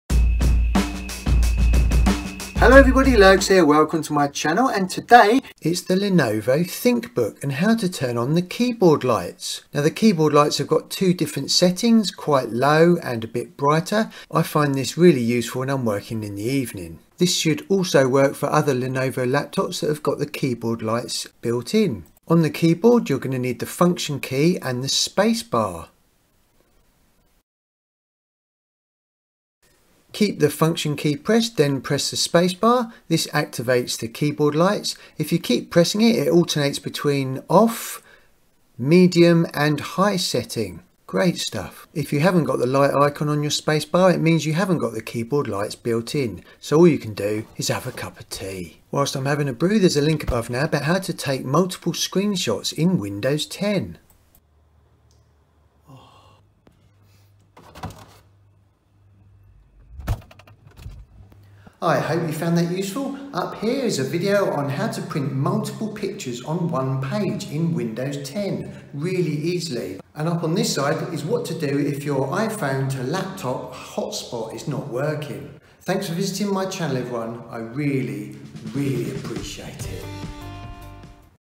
How To Turn on Keyboard light on a Lenovo Laptop - Lenovo Laptop keyboard light turn on
Turning on the backlight \ keyboard light on your Lenovo Laptop is very handy in the evening when it gets darker. It makes it much easier to see the Lenovo backlit keyboard.
In this video I will show you how to easily turn on or off your keyboard backlight on your Lenovo Laptop \ Thinkpad \ Thinkbook. And did you know it also has different Lenovo backlight settings.

Other useful How To Guides to help you save ⏱TIME and 💰 MONEY:
TECHNOLOGY